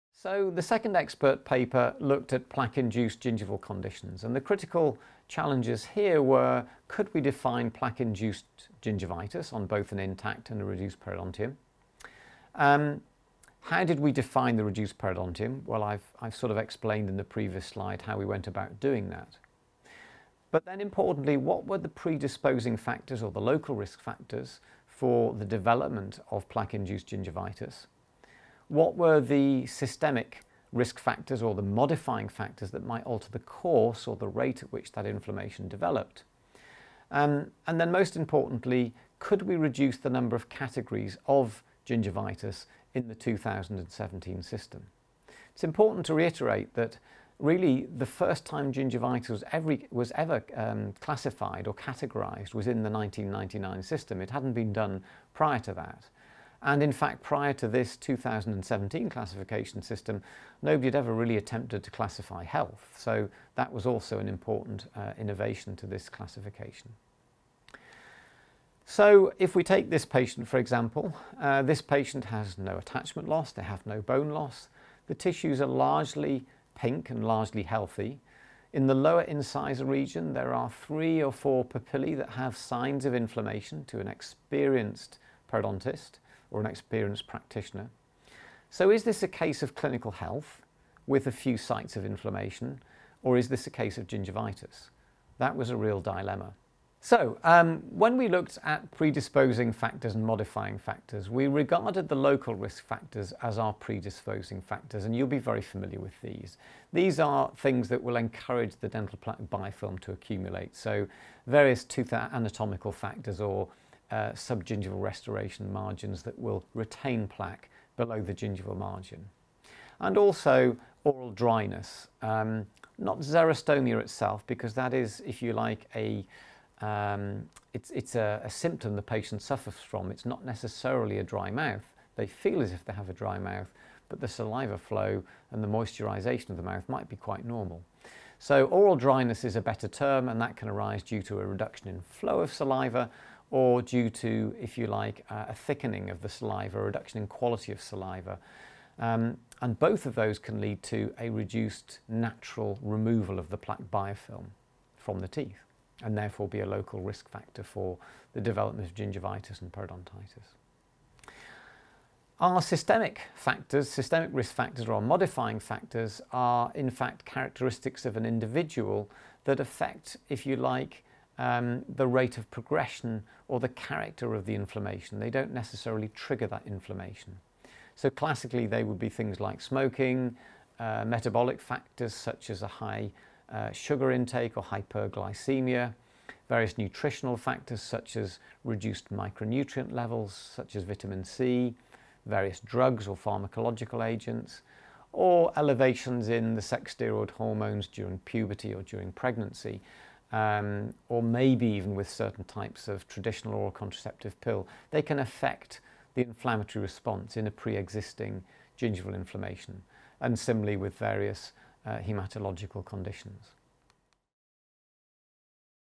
Plaque-induced gingival conditions - tutorial by Iain Chapple
EFPerio PerioWorkshop periodontology Video tutorial presented by Iain Chapple, summing up some of the 1st working group in World Perio Workshop 2017 on the new perio classification - in this case, plaque-induced gingival conditions. In particular, this workgroup focused on 'Periodontal health and gingival diseases and conditions'.